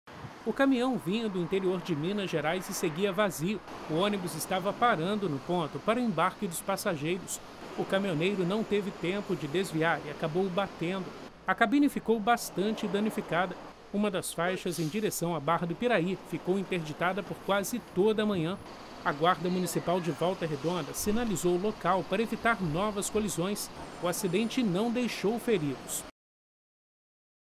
Lúcio Meira registra acidente entre ônibus e caminhão
Um ônibus e um caminhão se envolveram em acidente na Rodovia Lúcio Meira, em Volta Redonda.